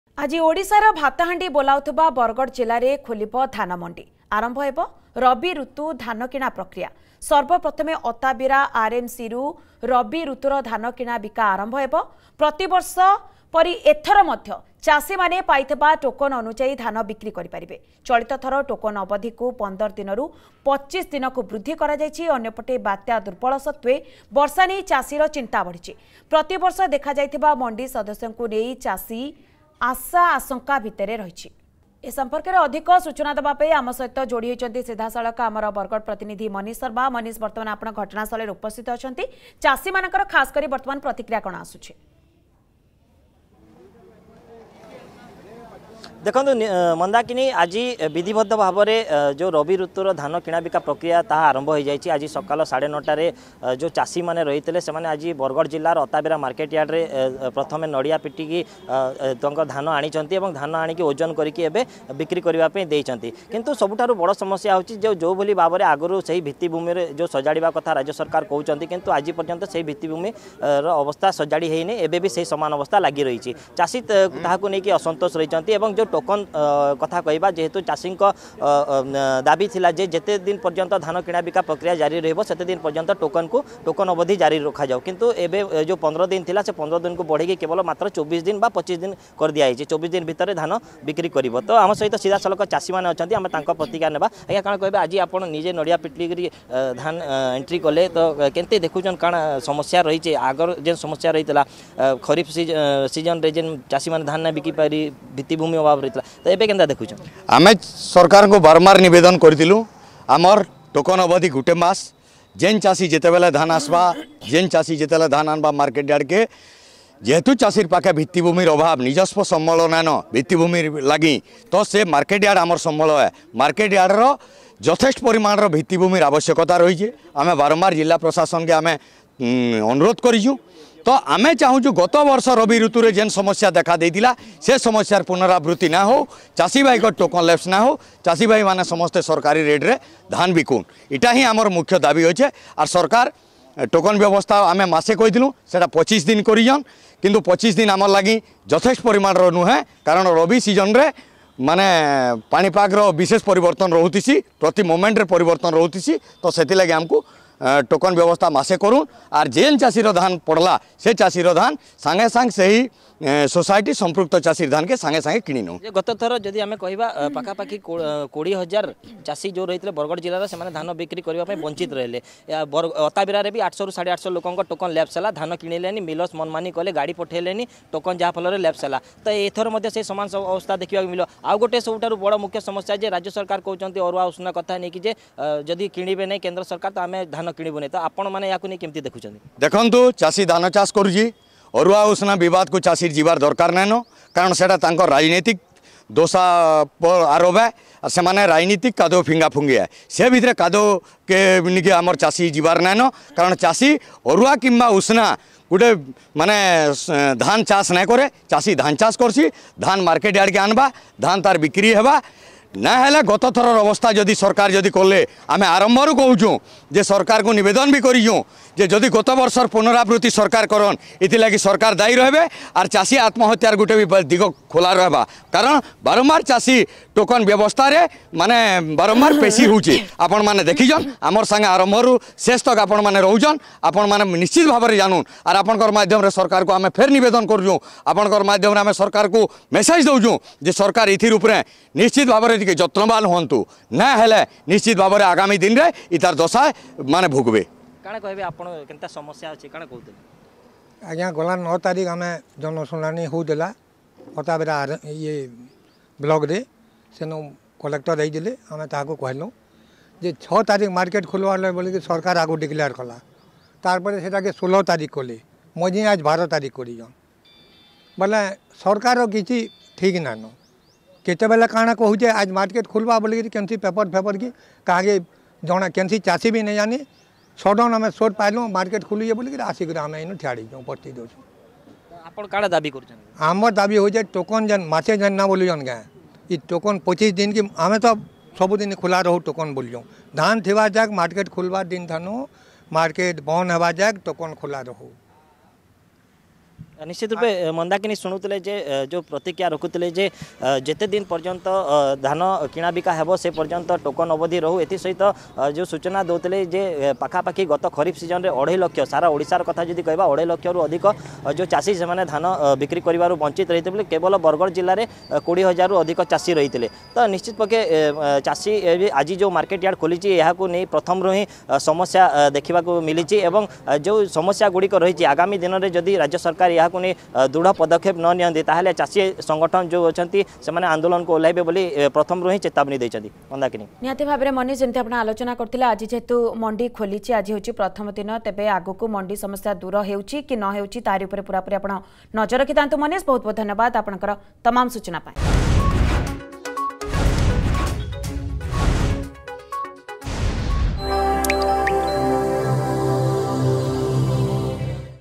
ଆଜି ବରଗଡ ଜିଲ୍ଲାରେ ଖୋଲିବ ଧାନ ମଣ୍ଡି
ଆଜି ଓଡ଼ିଶାର ଭାତ ହାଣ୍ଡି ବୋଲାଉଥିବା ବରଗଡ ଜିଲ୍ଲାରେ ଖୋଲିବ ଧାନ ମଣ୍ଡି । ଆରମ୍ଭ ହେବ ରବି ଋତୁର ଧାନ କିଣା ପ୍ରକ୍ରିୟା ।  ସର୍ବ ପ୍ରଥମେ ଅତାବିରା ଆର୍.ଏମ୍‌.ସିରୁ ରବି ଋତୁର ଧାନ କିଣା ବିକା ଆରମ୍ଭ ହେବ। ପ୍ରତିବର୍ଷ ପରି ଏଥର ମଧ୍ୟ ଚାଷୀମାନେ ପାଇଥିବା ଟୋକନ ଅନୁଯାୟୀ ଧାନ ବିକ୍ରି କରିବେ ।